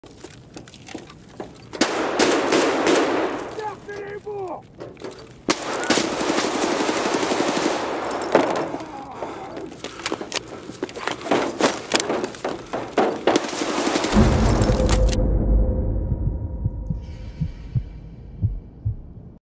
New Wipe - New Tagilla kills
Getting rocked by the infamous sledgehammer